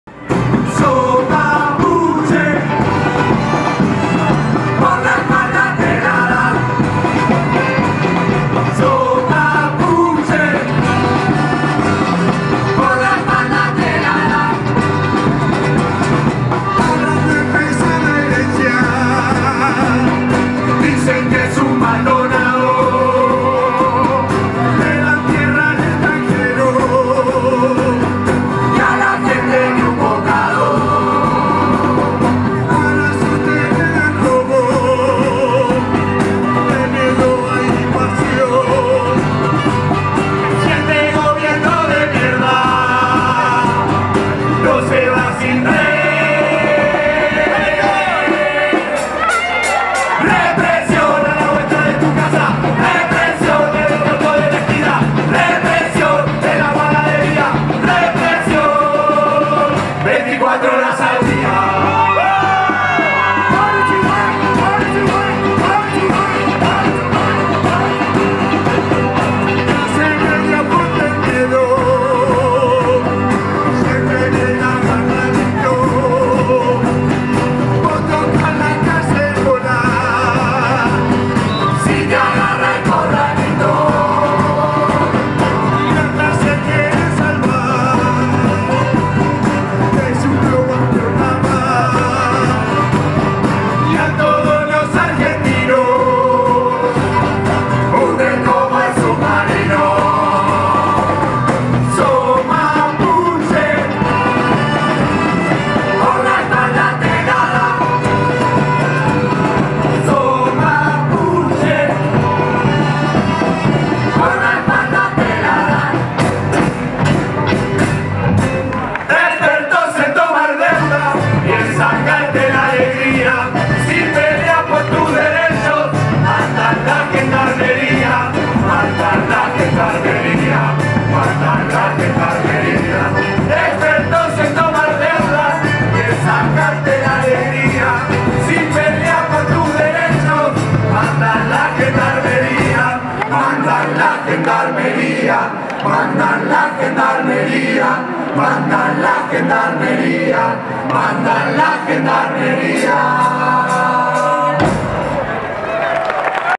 Atrevidos por Costumbre Murga Palermo Bs As --ph Fabian De Vita--
Atrevidos por Costumbre.
Murga del Barrio de Palermo.
Corso de Boedo .
Expertos en tomar deuda y en sacarte la alegría , si peleas por tus derechos mandan la gendarmeria .